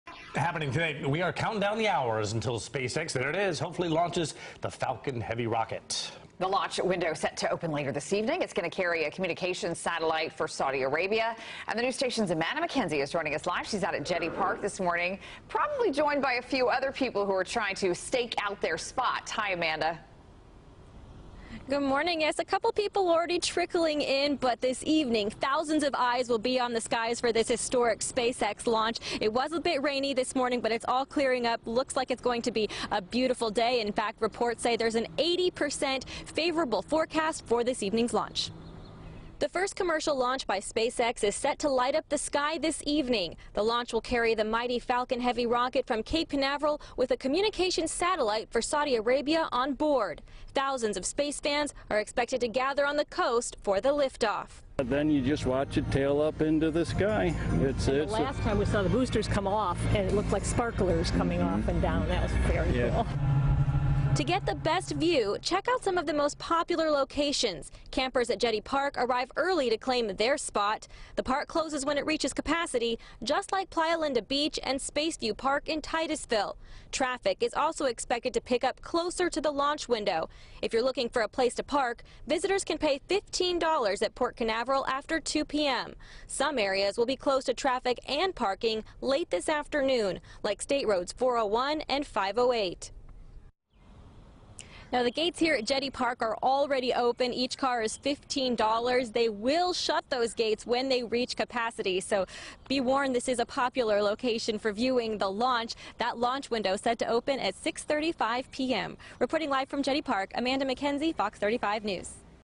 SpaceX set to launch Falcon Heavy rocket
SpaceX set to launch Falcon Heavy rocket on Wednesday night.